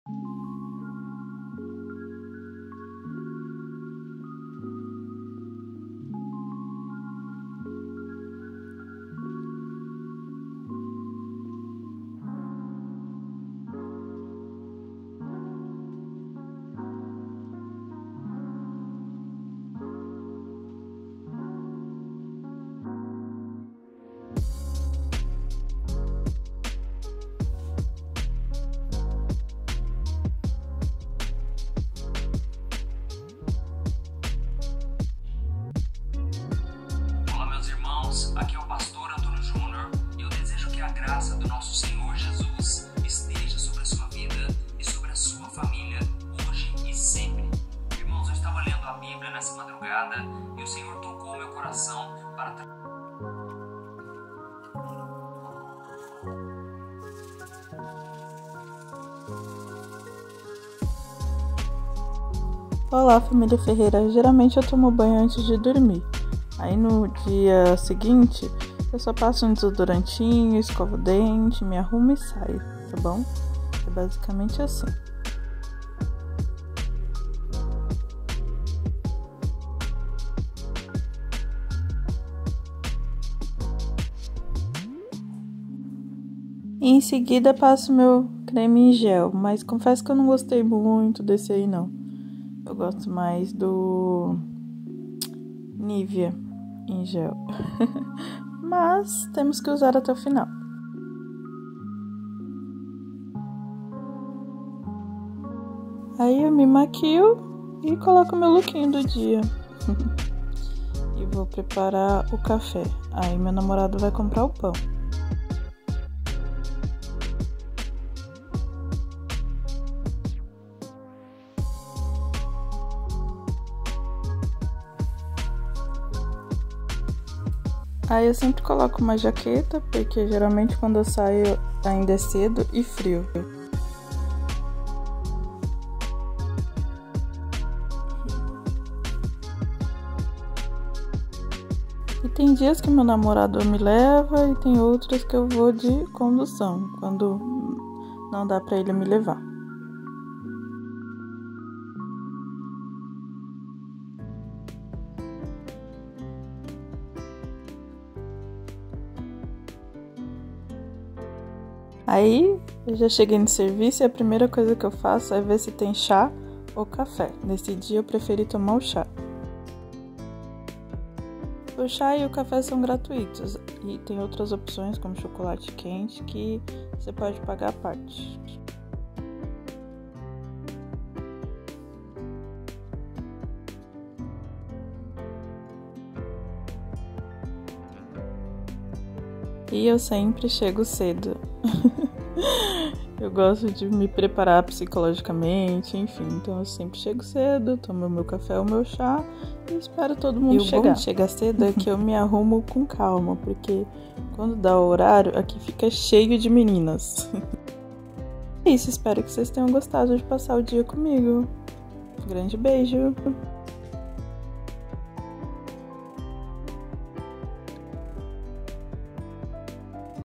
morandosozinha ROTINA DA MANHÃ TRABALHANDO FORA 🌸 vlog #2023
Olá família Ferreira tudo bem com vocês? Hoje trouxe um vídeo mostrando minha rotina da manhã trabalhando fora, lembrando que não posso mostrar onde trabalho, espero que gostem, grande bjo 😘❤️🌸